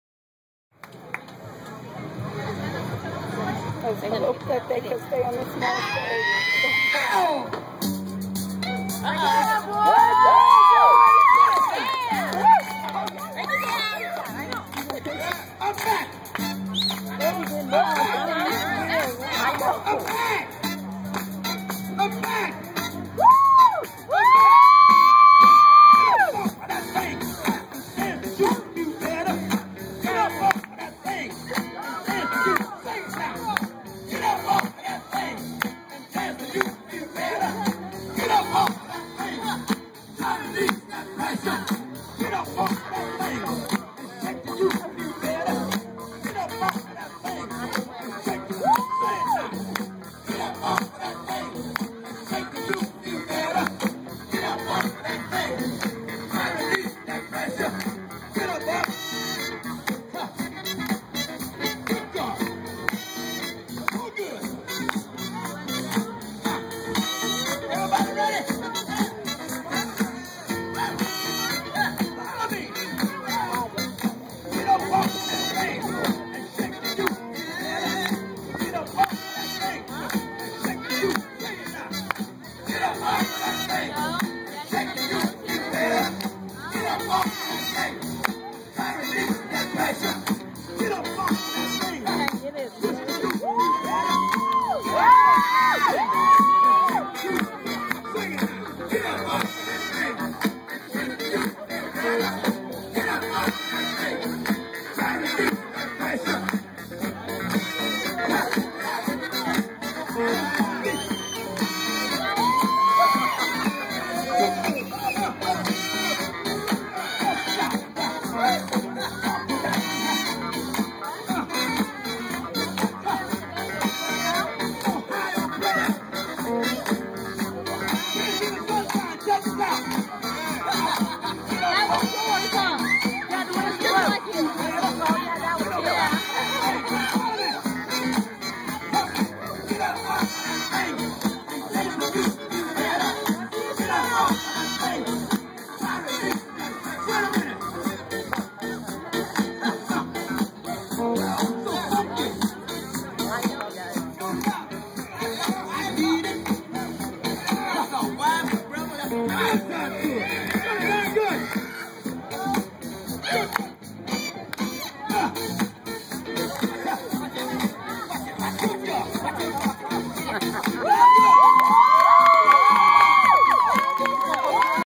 Dance - jazz 2012
Chrissy & peers from Joanna's Kids R It @ North Highlands Jubilee 2012